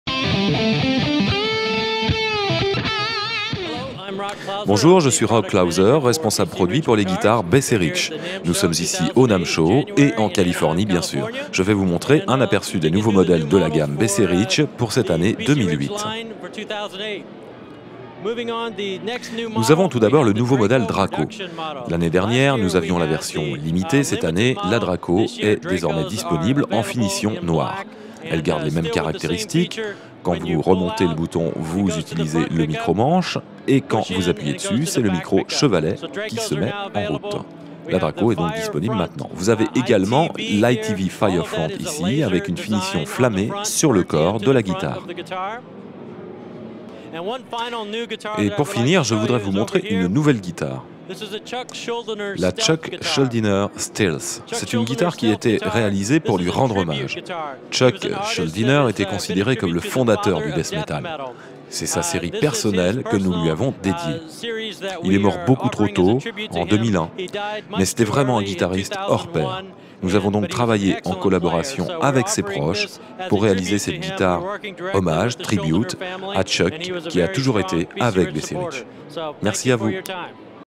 BC Rich au NAMM08 (La Boite Noire)
Voici la nouvelle gamme de guitares BC Rich plus métal que jamais !